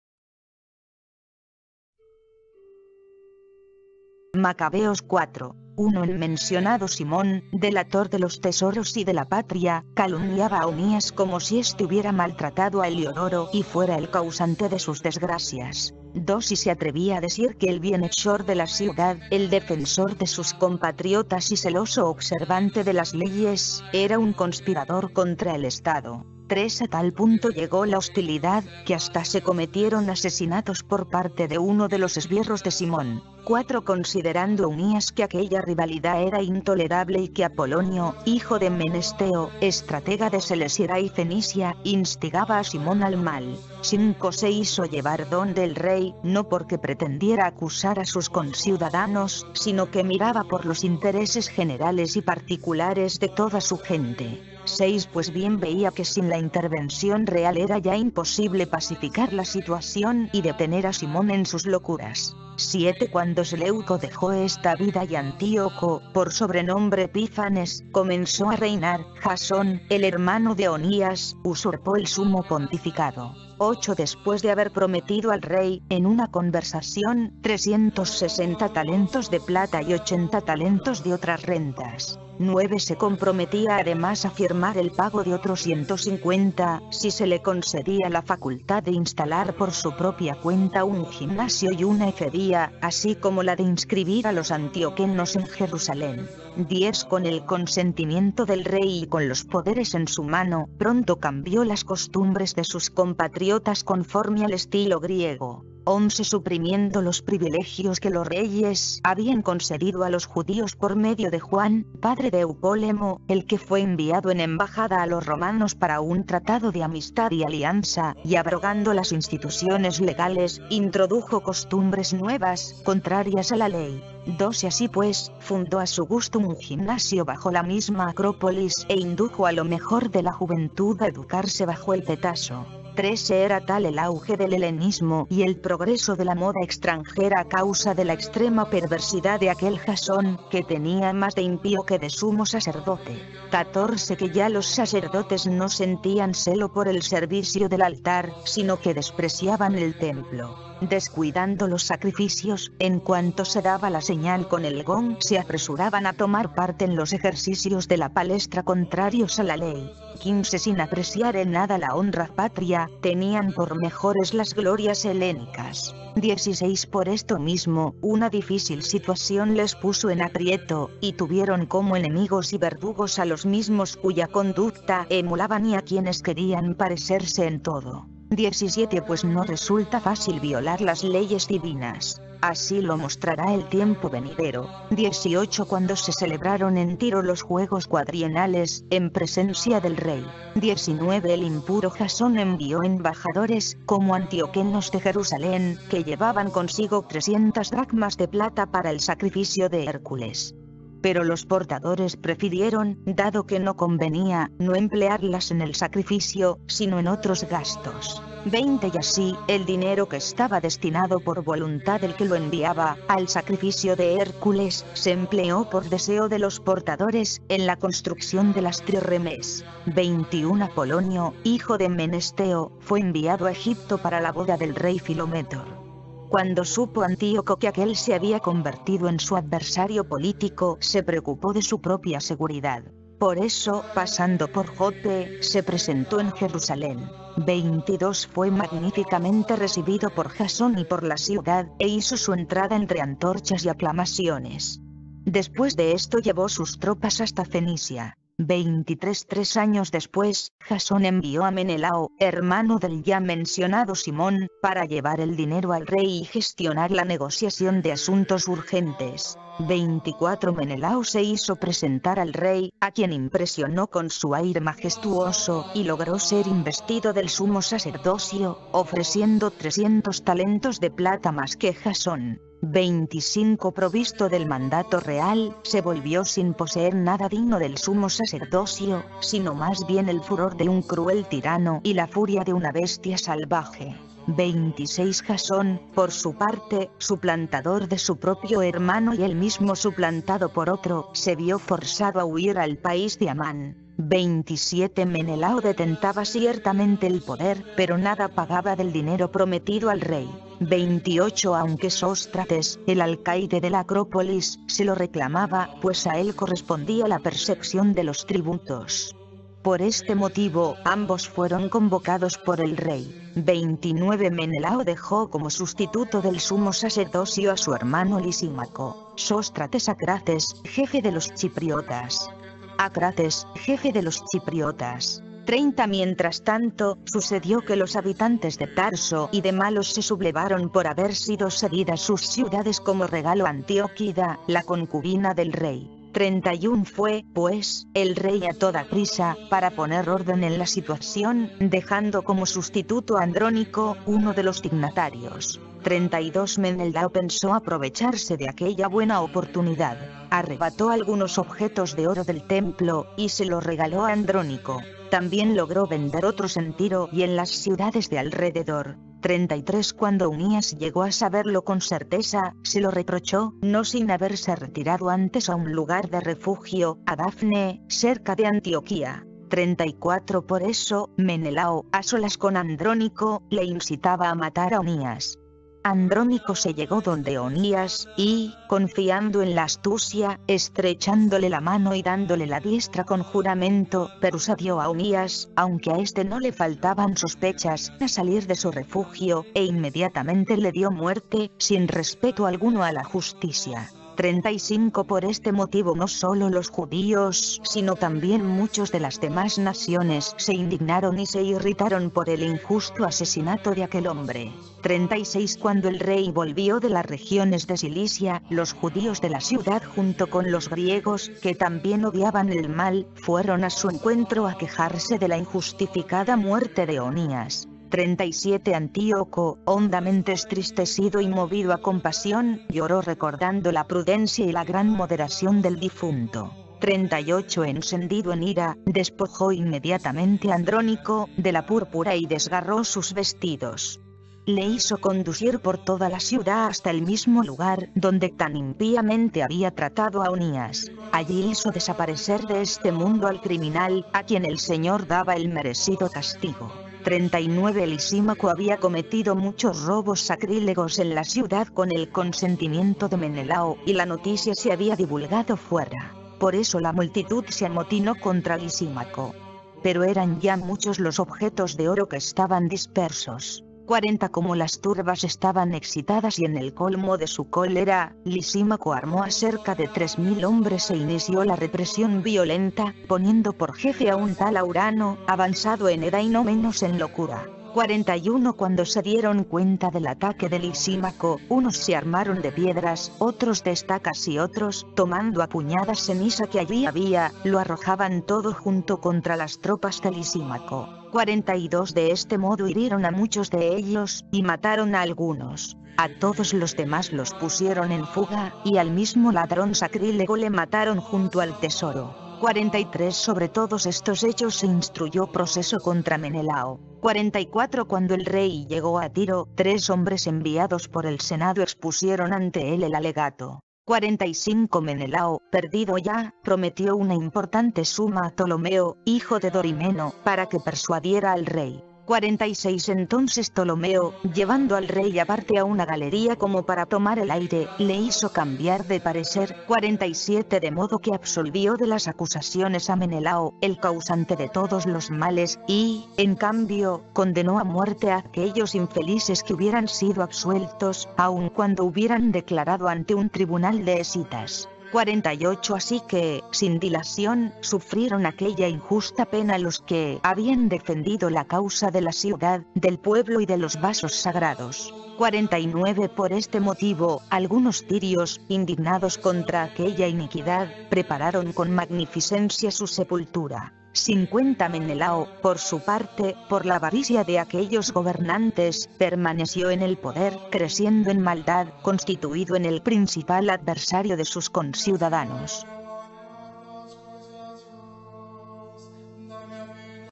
LA SANTA BIBLIA,VERSIÓN BIBLIA DE JERUSALÉN 1976, II Macabeos 4
II Macabeos 4

1 En mencionado Simón, delator de los tesoros y de la patria,
calumniaba a Onías como si éste hubiera maltratado a Heliodoro y fuera el
causante de sus desgracias;
2 y se atrevía a decir que el bienhechor de la ciudad, el defensor de
sus compatriotas y celoso observante de las leyes, era un conspirador contra
el Estado.
3 A tal punto llegó la hostilidad, que hasta se cometieron asesinatos
por parte de uno de los esbirros de Simón.
4 Considerando Onías que aquella rivalidad era intolerable y que
Apolonio, hijo de Menesteo, estratega de Celesira y Fenicia, instigaba a
Simón al mal,
5 se hizo llevar donde el rey, no porque pretendiera acusar a sus
conciudadanos, sino que miraba por los intereses generales y particulares de
toda su gente.
6 Pues bien veía que sin la intervención real era ya imposible
pacificar la situación y detener a Simón en sus locuras.
7 Cuando Seleuco dejó esta vida y Antíoco, por sobrenombre
Epífanes, comenzó a reinar, Jasón, el hermano de Onías, usurpó el sumo
pontificado,
8 después de haber prometido al rey, en una conversación, 360
talentos de plata y ochenta talentos de otras rentas.
9 Se comprometía además a firmar el pago de otro 150, si se le
concedía la facultad de instalar por su propia cuenta un gimnasio y una
efebía, así como la de inscribir a los Antioquenos en Jerusalén.
10 Con el consentimiento del rey y con los poderes en su mano,
pronto cambió las costumbres de sus compatriotas conforme al estilo
griego.
11 Suprimiendo los privilegios que los reyes habían concedido a los
judíos por medio de Juan, padre de Eupólemo, el que fue enviado en
embajada a los romanos para un tratado de amistad y alianza, y abrogando
las instituciones legales, introdujo costumbres nuevas, contrarias a la Ley.
12 Así pues, fundó a su gusto un gimnasio bajo la misma acrópolis e
indujo a lo mejor de la juventud a educarse bajo el petaso.
13 Era tal el auge del helenismo y el progreso de la moda extranjera a
causa de la extrema perversidad de aquel Jasón, que tenía más de impío que
de sumo sacerdote,
14 que ya los sacerdotes no sentían celo por el servicio del altar, sino
que despreciaban el Templo; descuidando los sacrificios, en cuanto se daba
la señal con el gong se apresuraban a tomar parte en los ejercicios de la
palestra contrarios a la ley;
15 sin apreciar en nada la honra patria, tenían por mejores las glorias
helénicas.